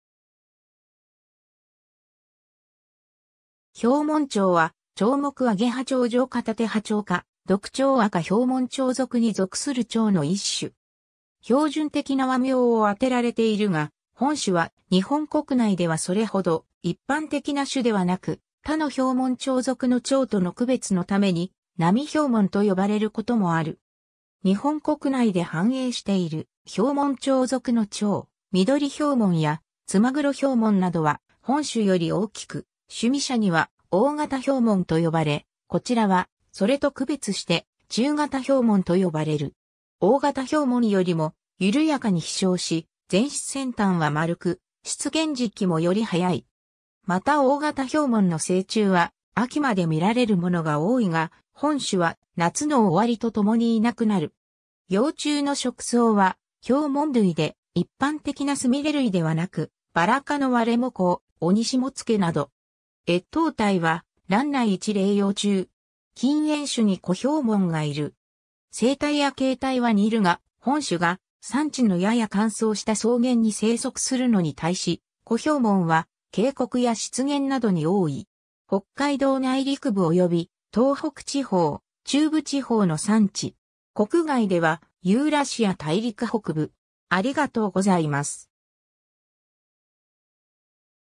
ヒョウモンチョウ
ヒョウモンチョウ（豹紋蝶、 Brenthis daphne）は、チョウ目（鱗翅目）アゲハチョウ上科タテハチョウ科ドクチョウ亜科ヒョウモンチョウ族に属するチョウの一種。
 標準的な和名をあてられているが、本種は日本国内ではそれほど一般的な種ではなく、他のヒョウモンチョウ族の蝶との区別のために「ナミヒョウモン」と呼ばれることもある。
日本国内で繁栄しているヒョウモンチョウ族のチョウ、ミドリヒョウモンやツマグロヒョウモンなどは本種より大きく、趣味者には「大型ヒョウモン」と呼ばれ、こちらはそれと区別して「中型ヒョウモン」と呼ばれる。
 大型ヒョウモンよりもゆるやかに飛翔し、前翅先端は丸く、出現時期もより早い。
また大型ヒョウモンの成虫は秋まで見られるものが多いが、本種は夏の終わりとともにいなくなる。
 幼虫の食草はヒョウモン類で一般的なスミレ類ではなく、バラ科のワレモコウ、オニシモツケなど。
越冬態は卵内1齢幼虫。
 近縁種にコヒョウモン（B. ino）がいる。
生態や形態は似るが、本種が山地のやや乾燥した草原に生息するのに対し、コヒョウモンは渓谷や湿原などに多い。
 北海道内陸部および、東北地方・中部地方の山地。
国外ではユーラシア大陸北部。